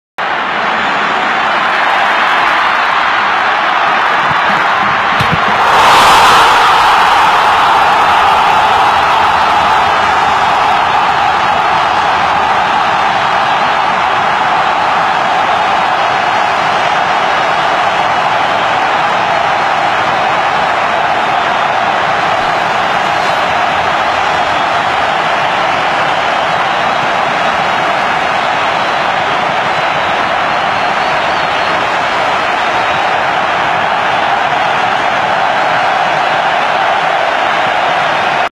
Hoy cumple años la magia del fútbol... ¡Muchas felicidades Ronaldinho Gaúcho!
Hoy cumple años la magia del fútbol...

¡Muchas felicidades Ronaldinho Gaúcho!